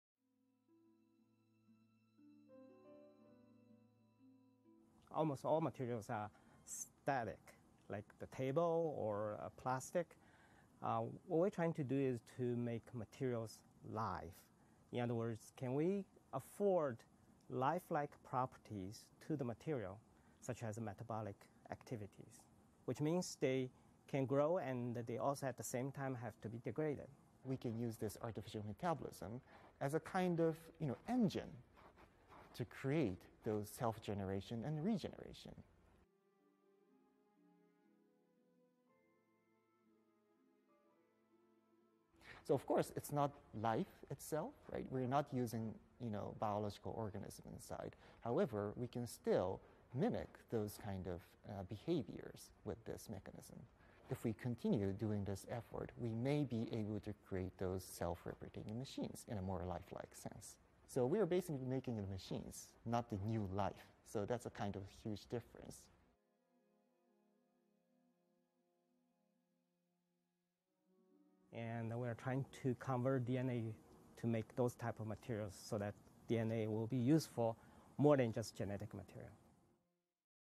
Engineers tap DNA to create 'lifelike' machines
||| Cornell professor of biological and environmental engineering Dan Luo and research associate Shogo Hamada have created a DNA material capable of metabolism, in addition to self-assembly and organization.